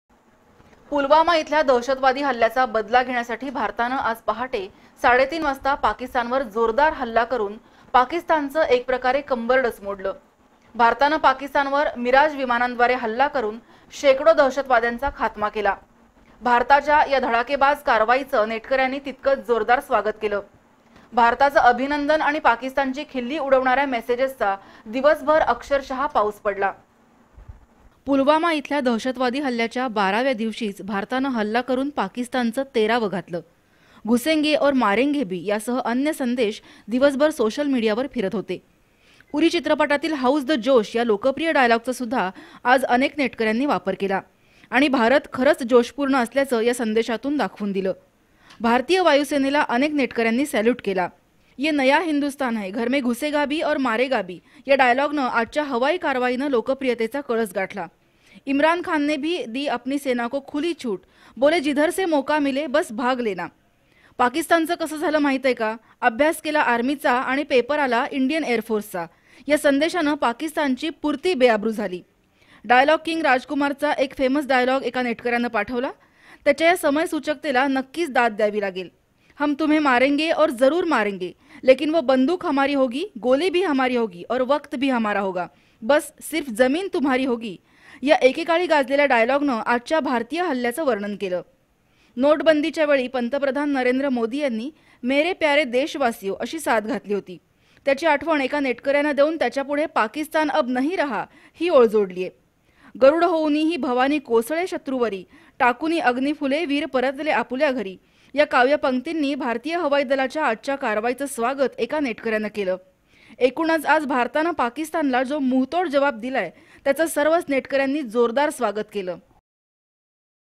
भारतानं सीमेपलिकडं जावून केलेल्या धडाकेबाज कारवाईचं नेटकर्‍यांकडून जोरदार स्वागत
भारतानं सीमेपलिकडं जावून केलेल्या धडाकेबाज कारवाईचं नेटकर्‍यांकडून जोरदार स्वागत, दिवसभर सोशल मिडियावर सैनिकांचं अभिनंदन आणि पाकिस्तानची खिल्ली उडवणार्‍या मेसेजचा पाऊस
Channel B